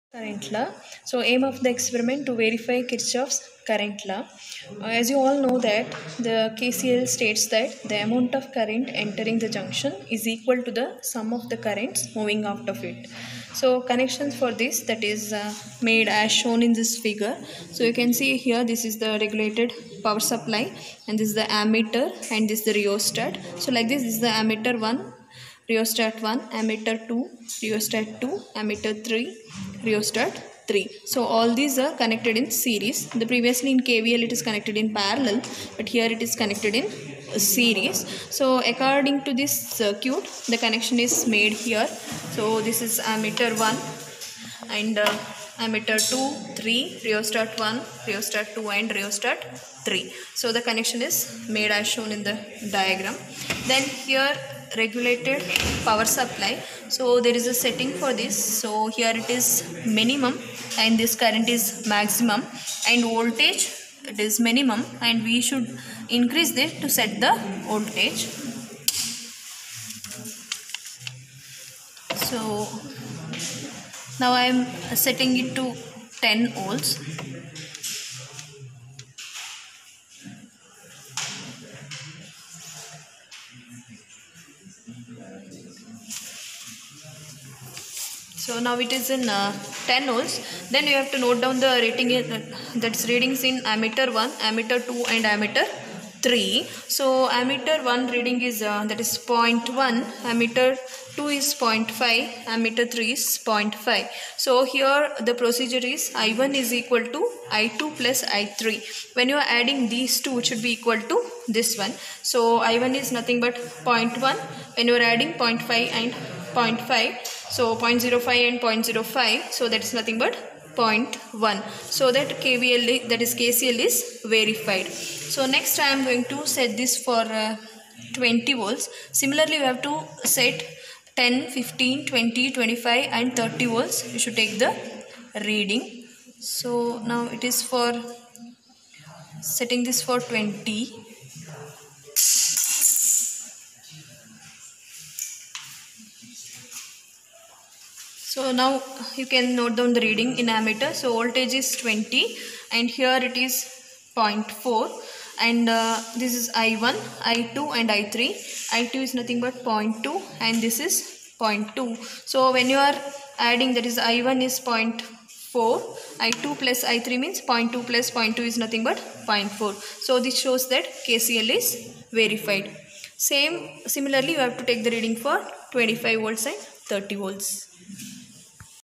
KIRCHOFF,S CURRENT LAW, Basic Electrical Lab Experiment
KCL experiment for first year B.E students
By Professor Sushma KR
Dept of ECE